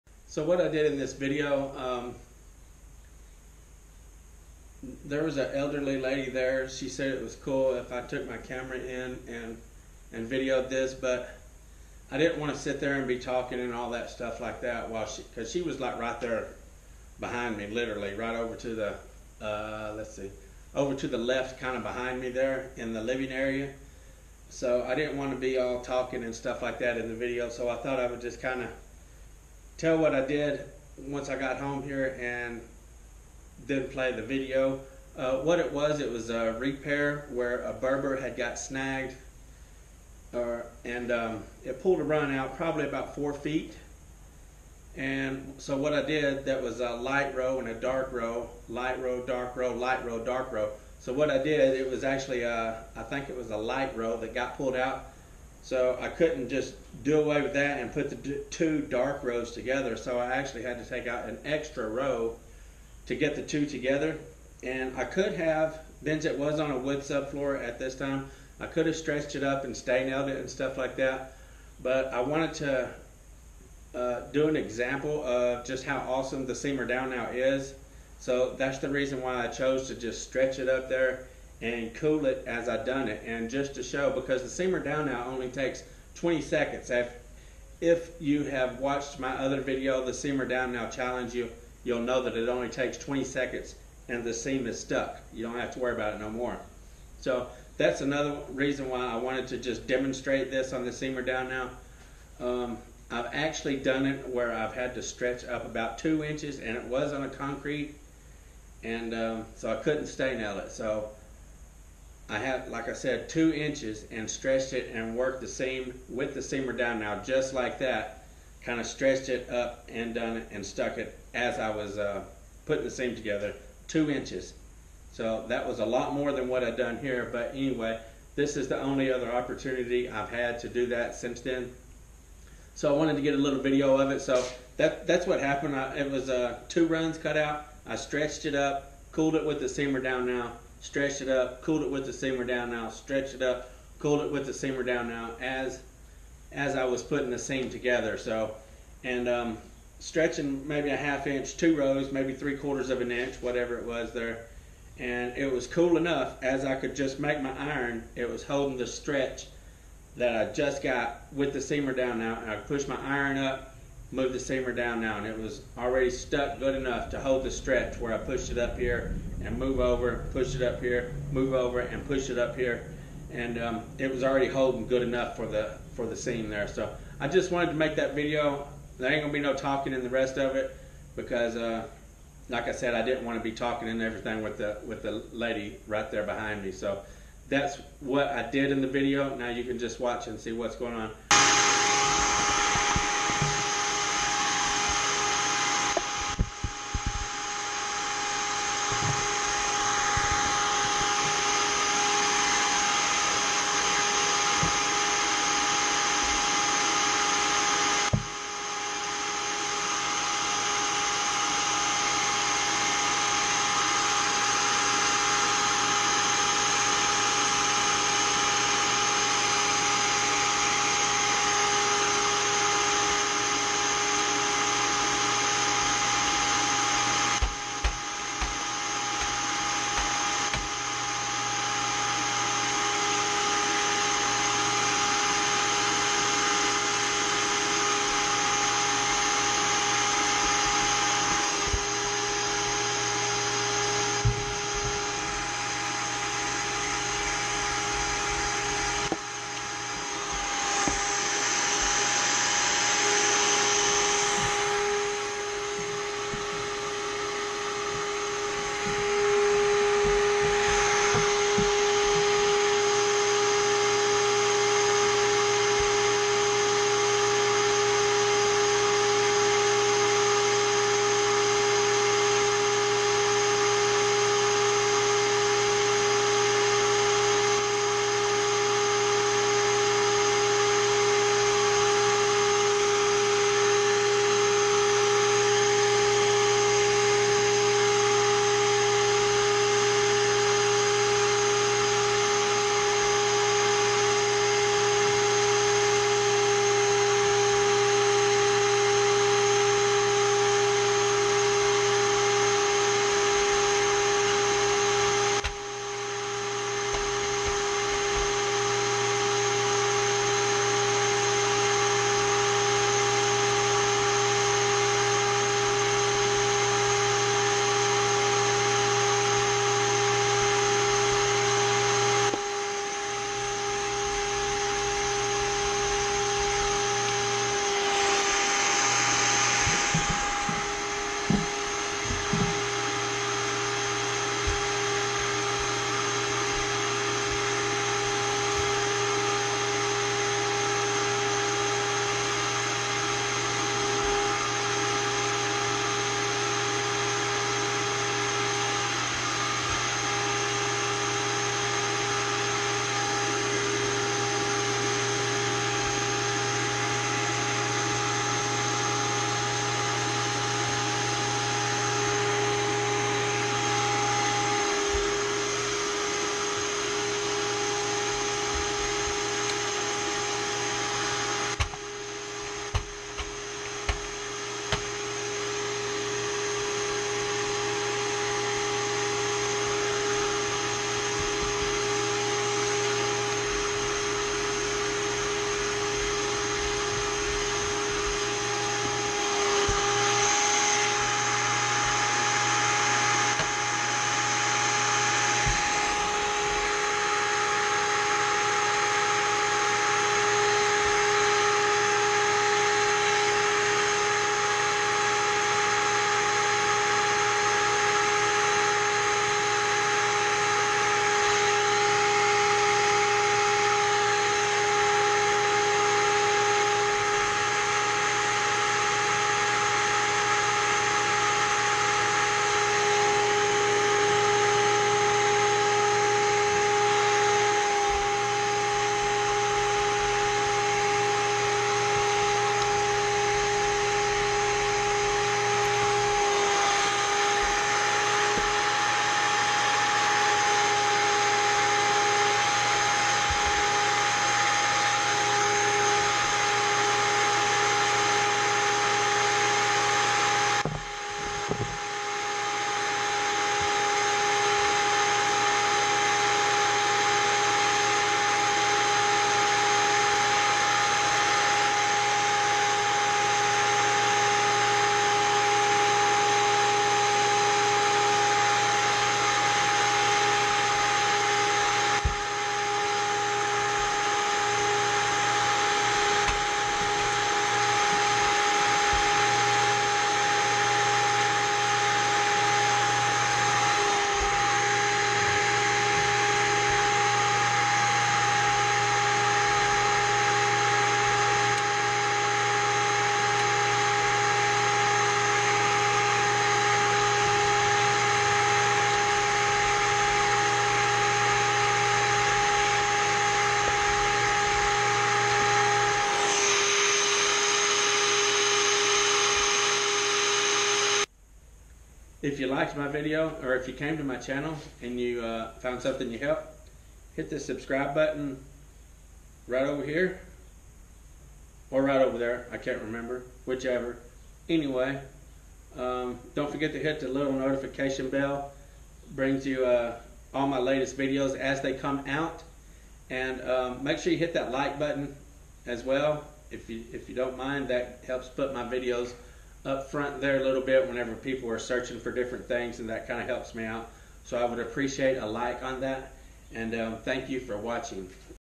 BERBER REPAIR
an excellent demonstration showing how awesome the Seamer Down Now is